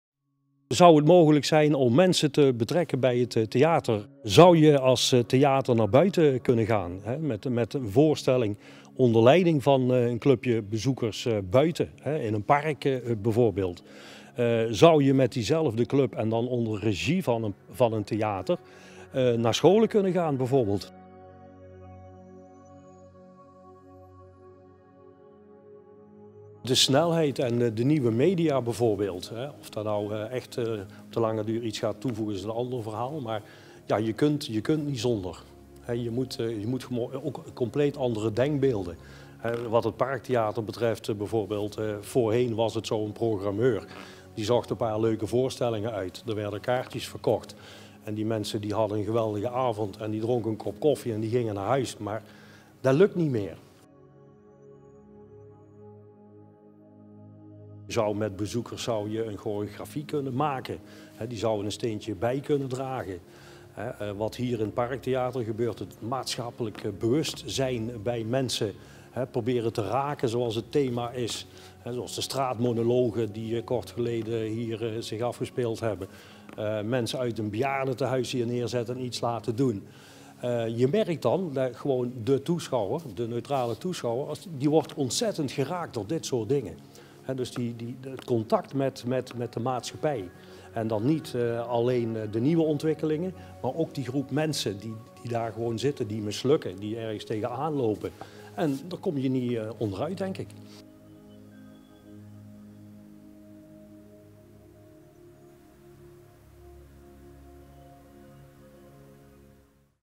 Léon van der Zanden
Parktheater Eindhoven 2020
Parktheater Eindhoven heeft in het kader van het 50-jarig jubileum in oktober 2014, vijftig mensen gevraagd hun vergezicht te schetsen van de toekomst in 2020. Zij spreken over hun vakgebied en de rol die zij het Parktheater dan toedichten.
Aan het woord: Leon van der Zanden - Pionier en Parktheater-genieter.